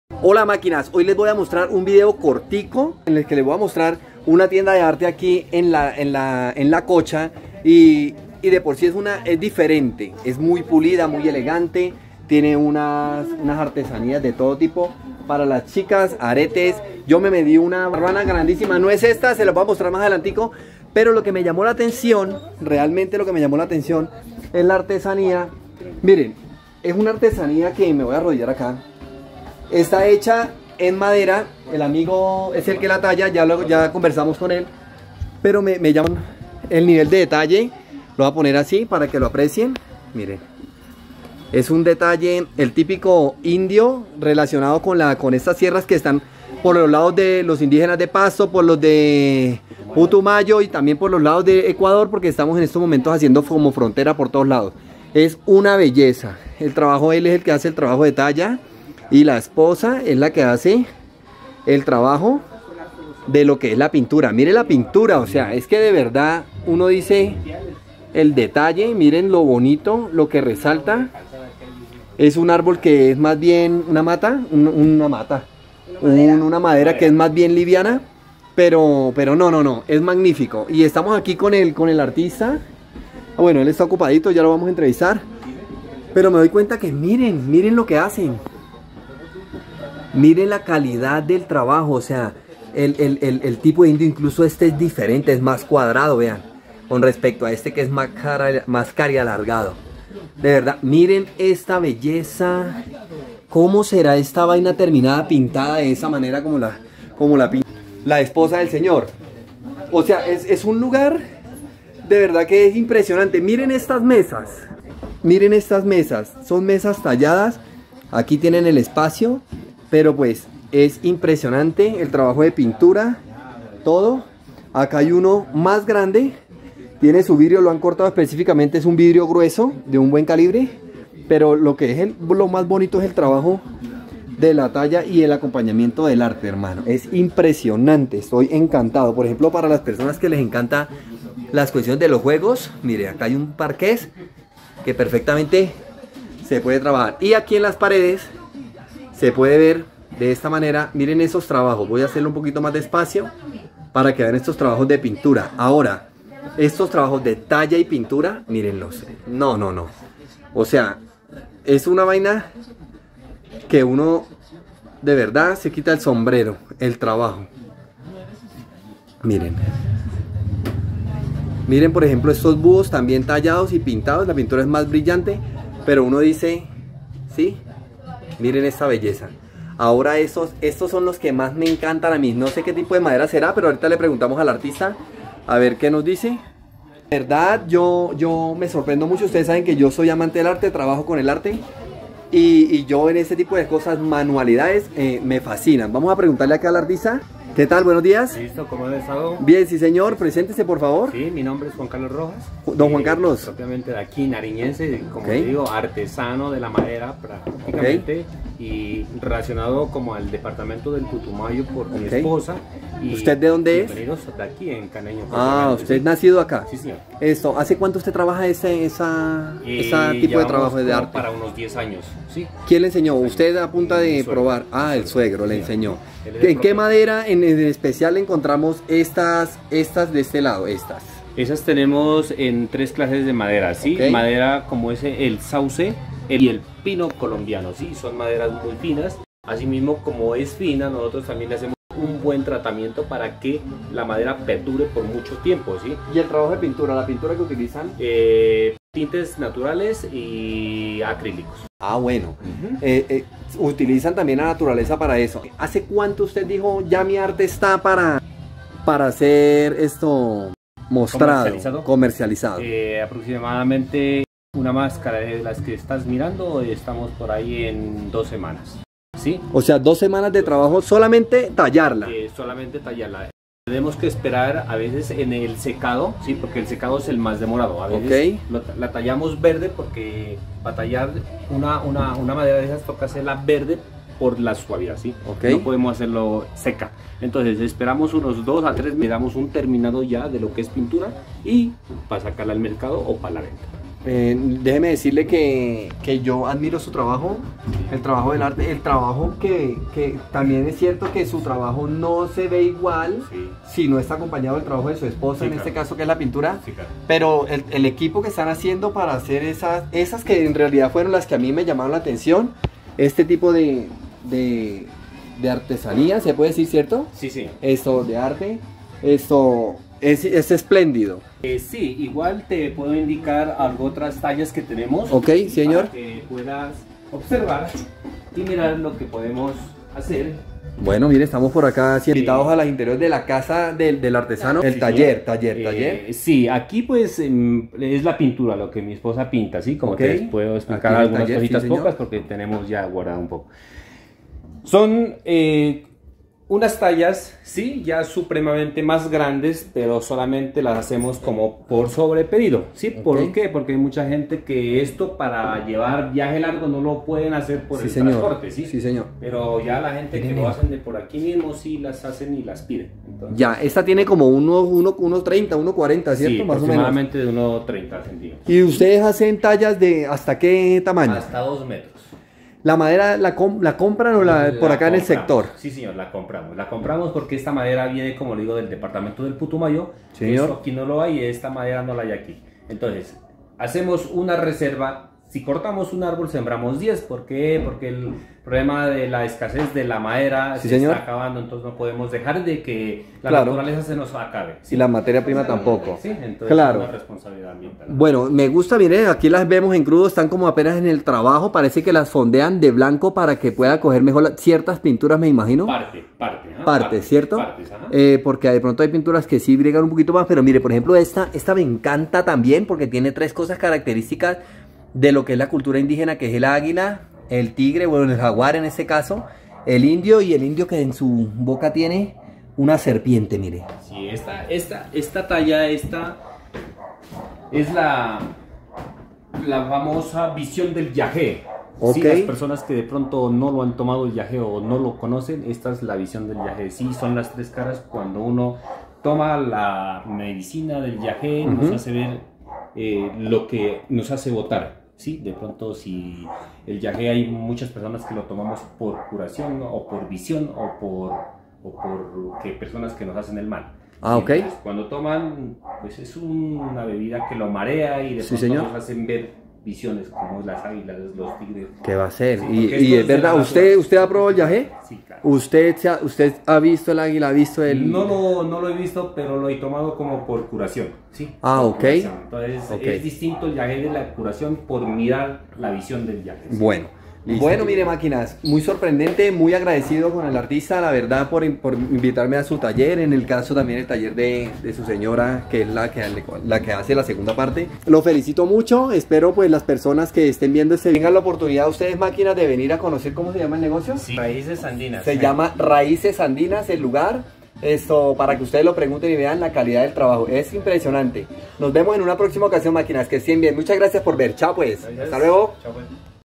Artesanías asombrosas en la Laguna de la Cocha - Pasto Nariño
Un lugar lindo que encontré, donde aprecié el arte que realiza una pareja, uno talla la madera y la otra la pinta, de verdad me gusto mucho, espero les guste!

Si desea acompañamiento de turismo